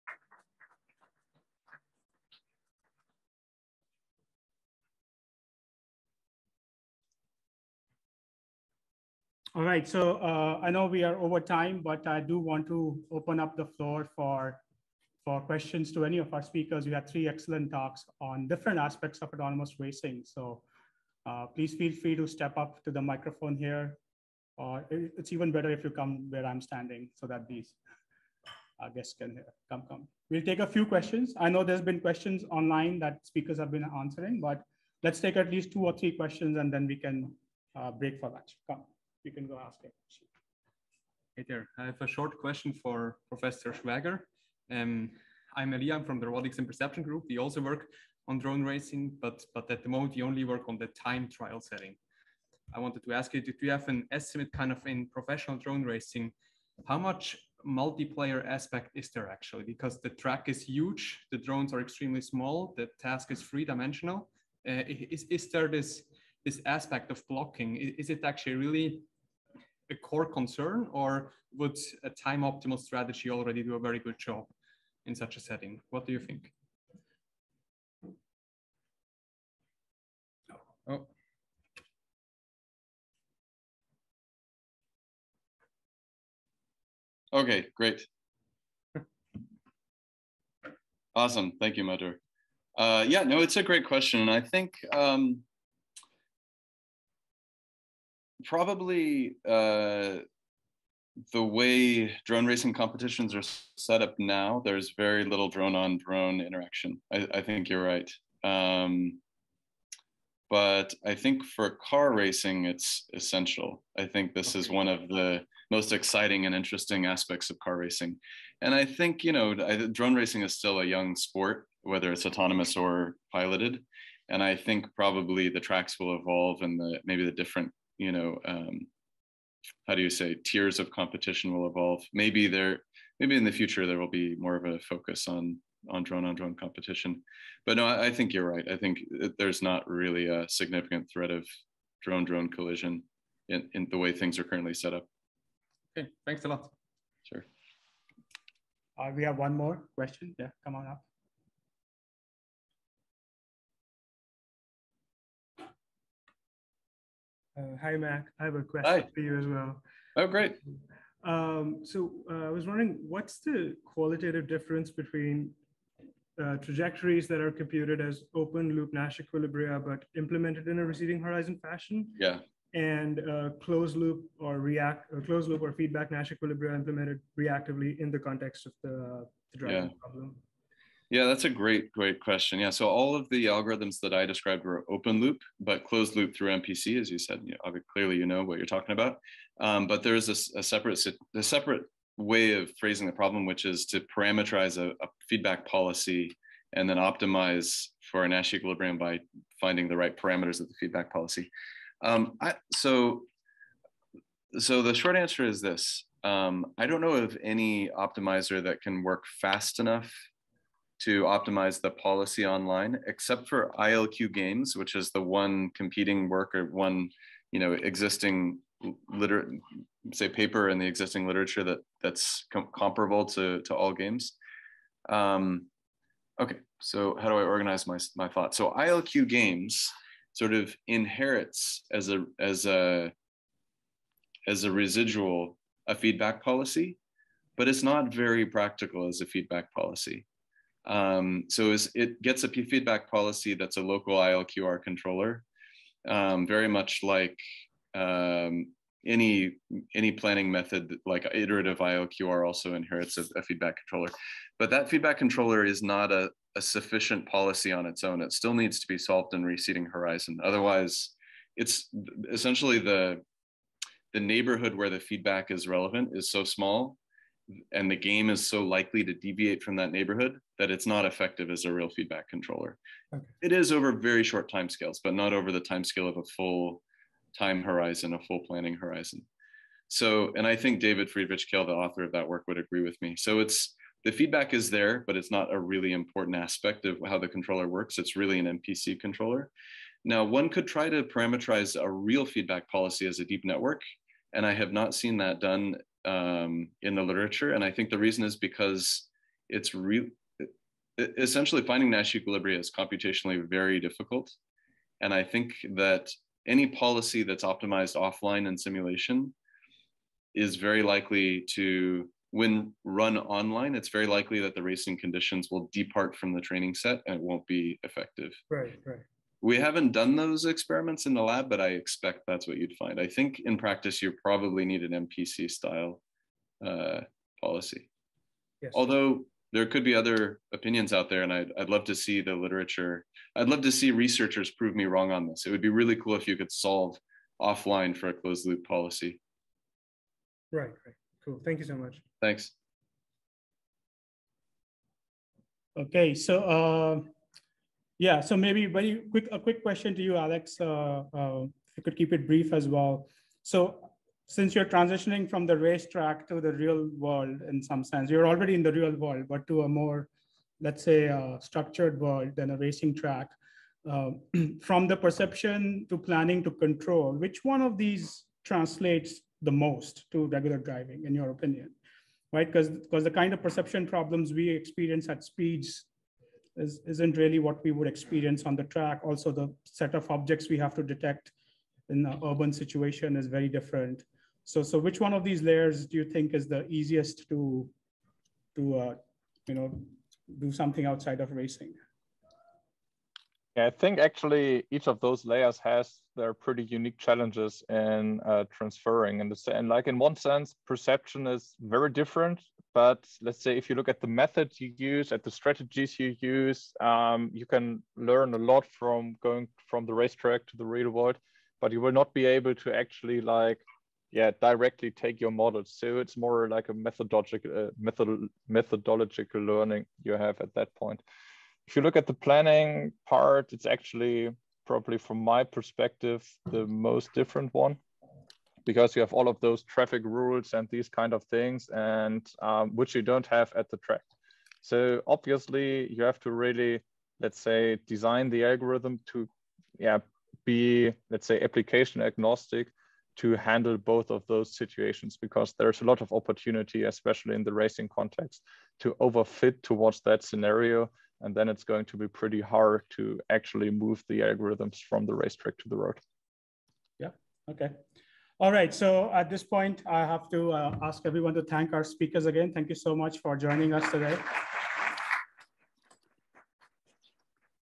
ICRA 2022 Autonomous Racing - Q&A Invited Speakers Session 2
Invited Session 2 - Q&A
Marko Bertogna (University of Modena)
Mac Schwager (Stanford University)
Alexander Wischnewski (driveblocks)

From the ICRA 2022 Full-Day Workshop on Opportunities and Challenges with Autonomous Racing.

Workshop Chairs:
Johannes Betz (University of Pennsylvania)
Madhur Behl (University of Virginia)
Rahul Mangharm (University of Pennsylvania)
Venkat Krovi (Clemson University)